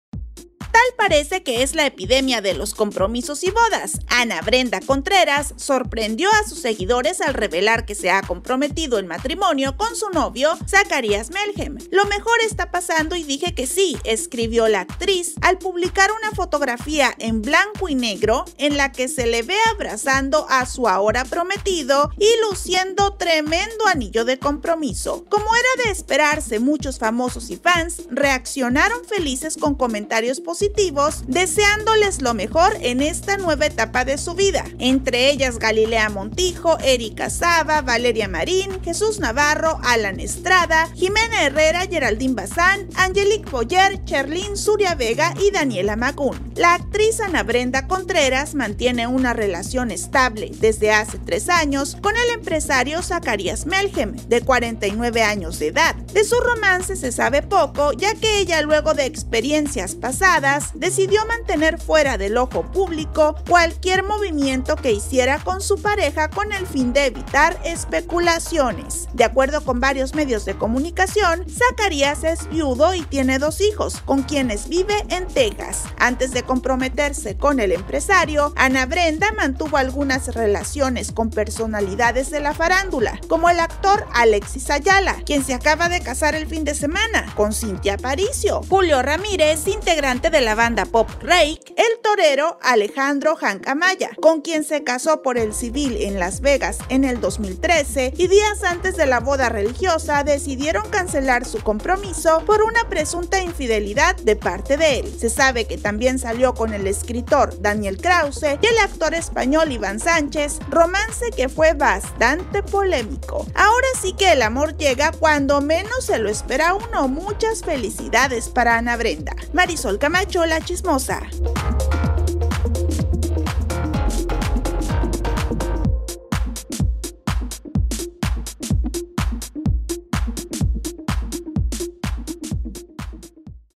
🔵 Ana Brenda Contreras SE CASARÁ POR SEGUNDA VEZ❗ SERÁ MADRASTRA
AnaBrendaContreras se compromete en matrimonio.